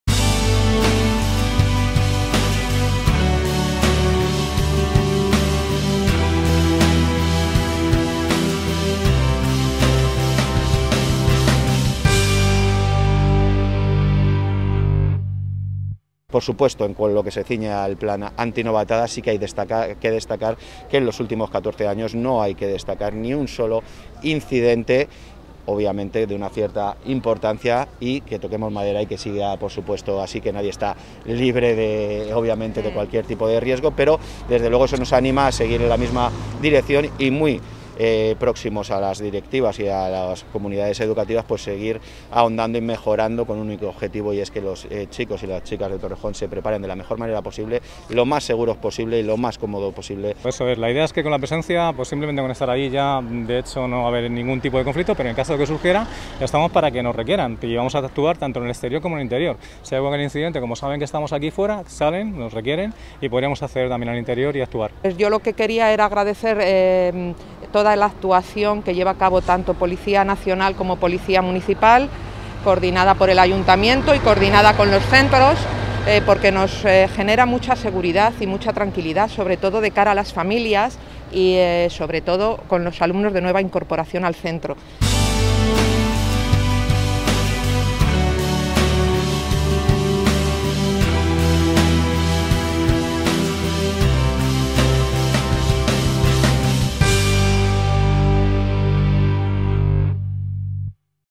Torrejón de Ardoz logra erradicar las gamberradas más graves con su plan antinovatadas
El objetivo de este dispositivo policial es la prevención de novatadas y acoso escolar a los nuevos alumnos de los institutos de la ciudad.